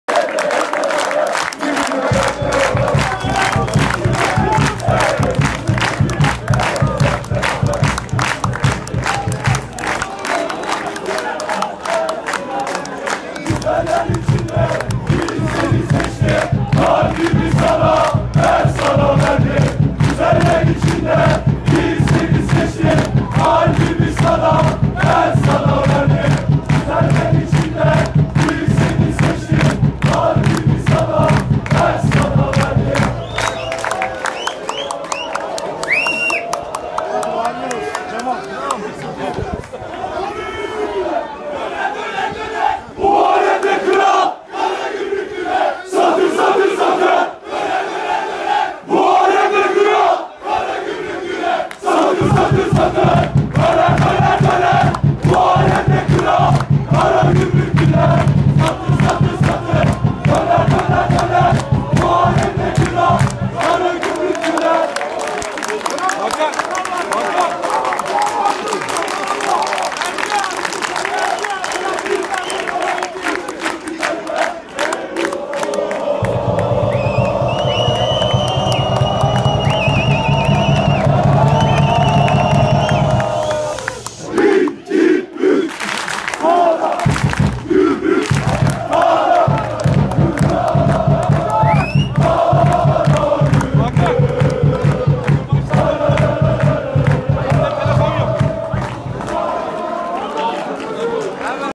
Karagümrük-Niğde Belediyespor Maç Sonu Galibiyet Sevinci-Satır Döner, Emir Gökçe'nin Üçlüsü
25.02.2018 tarihinde Karagümrük - Niğde Belediyespor maç sonundan görüntüler.